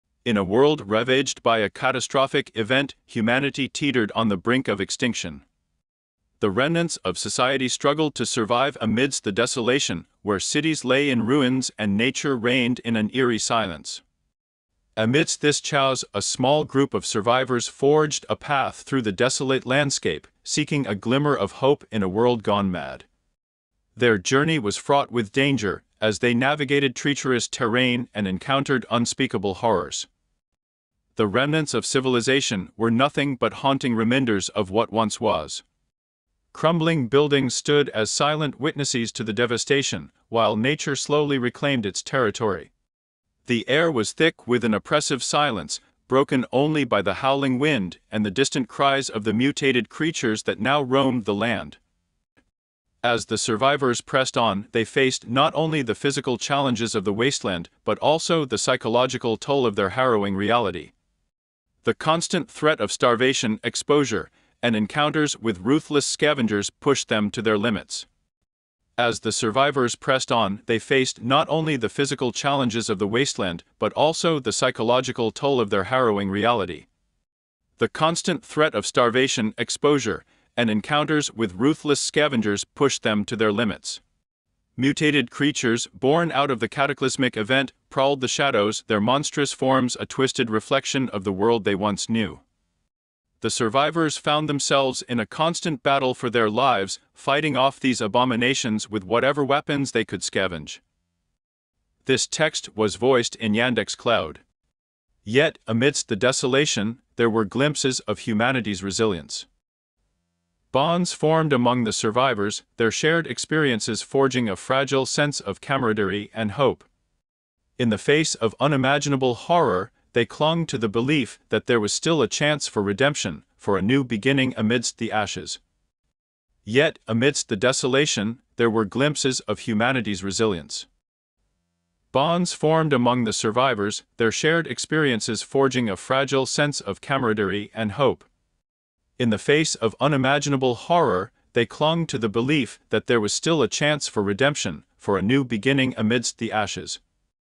Desolation's Path - A Post Apocalyptic
Desolation's Path follows the survivors as they navigate a treacherous journey through a post-apocalyptic landscape, facing the horrors of the wasteland and the moral dilemmas that arise in the fight for survival. Will they find salvation in a world ravaged by despair, or will they succumb to the desolation that surrounds them?